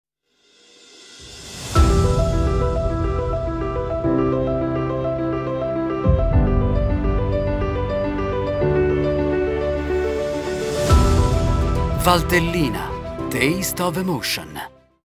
Inverno in Valtellina tra sport e natura
Gusta l’emozione di paesaggi rigeneranti e vivi una natura autentica, tra sport e attività adatte a tutta la famiglia. Scopri di più su

What to do in Valtellina during winter among sport and amazing nature!